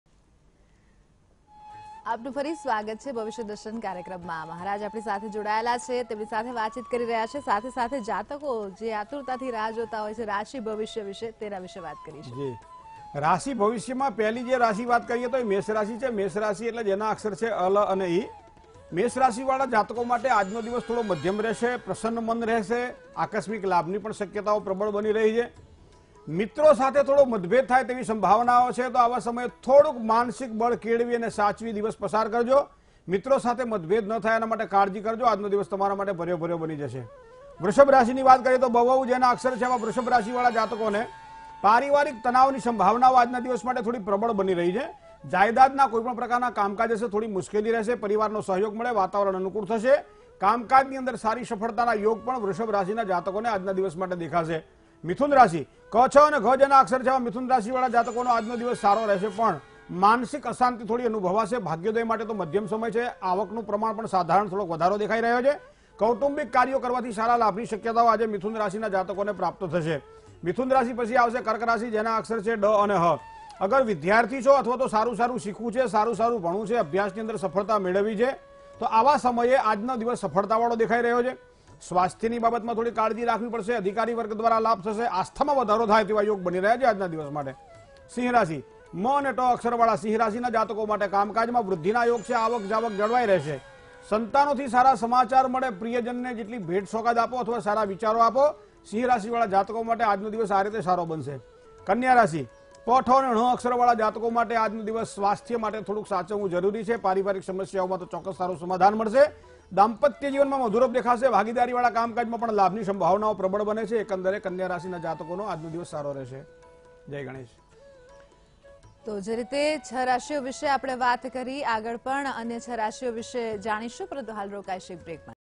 Bhavishya Darshan | જાણો 5th December'18 Panchang શું છે? કેવો રહેશે તમારો આજનો દિવસ | Vtv News
Bhavishya Darshan | જાણો 5th December'18 Panchang શું છે? કેવો રહેશે તમારો આજનો દિવસ.